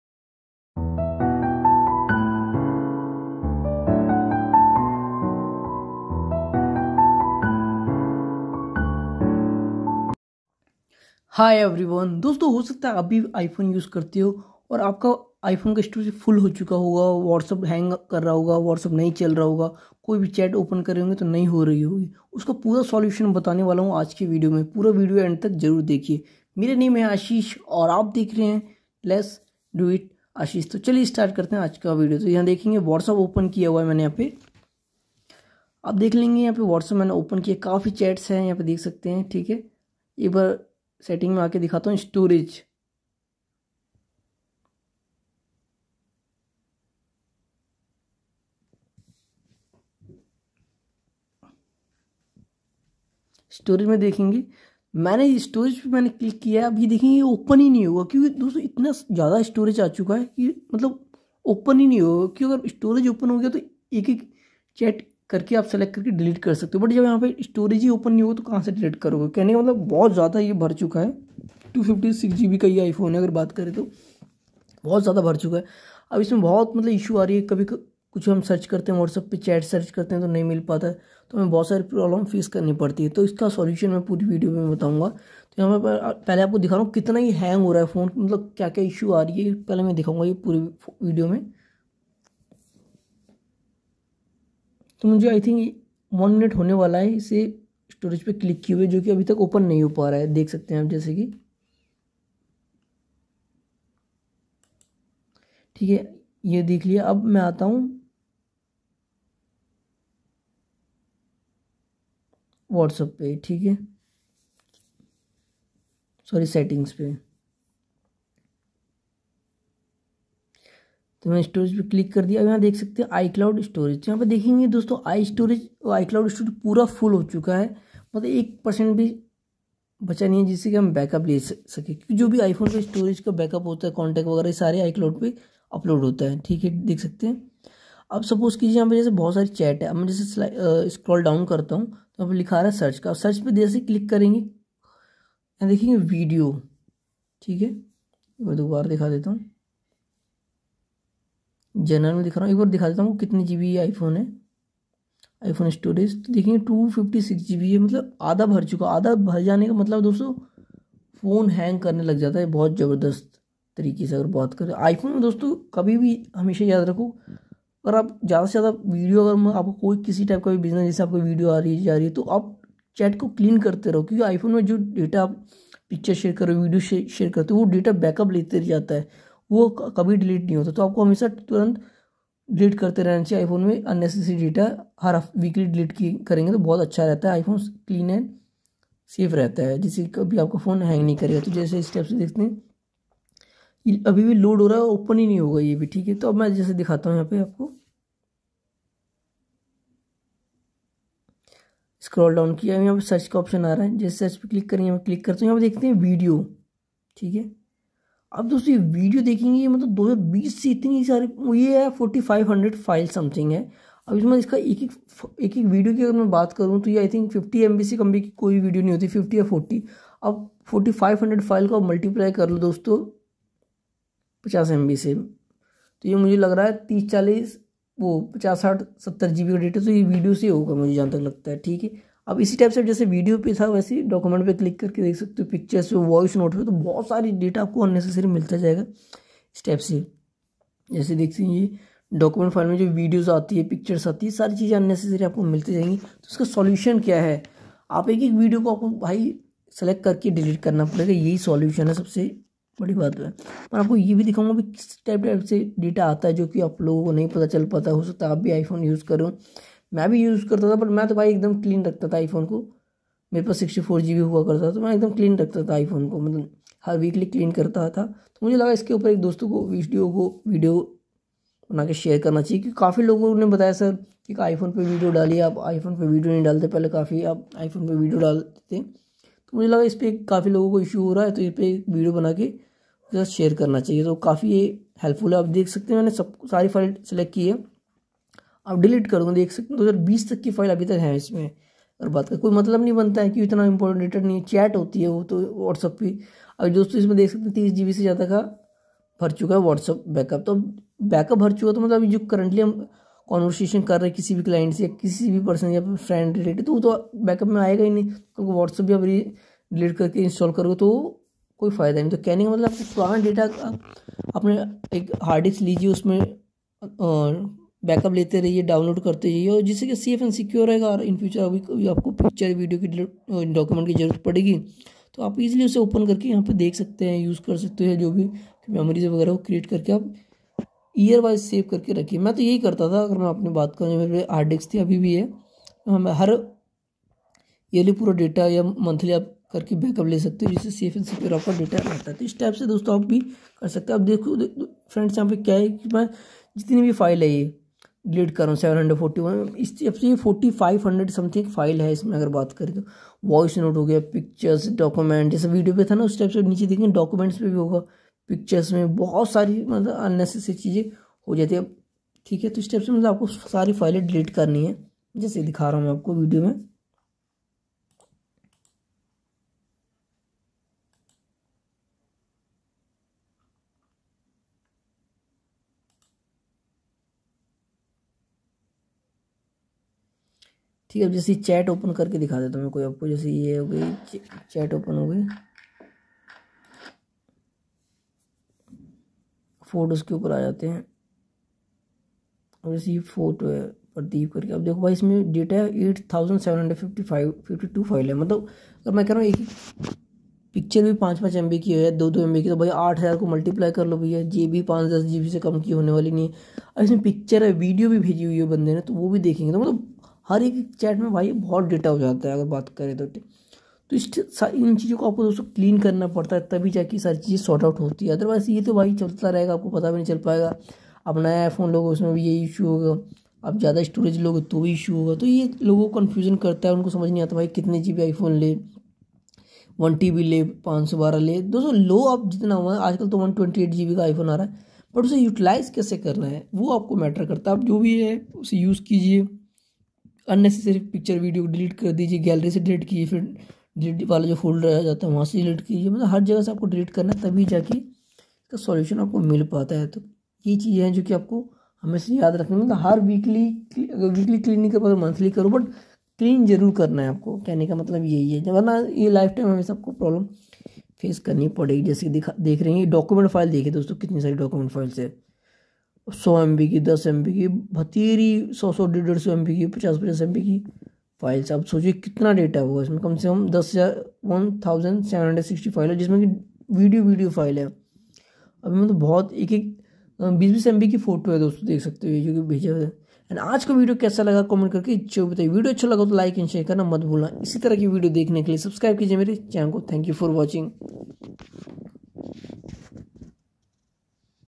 Whatsapp Not Working On Iphone Because Storage Is Full? Here's What To Do - Letsdoitashish
Aaj ki video main maine btaya hain kaise iphone storage clear kare.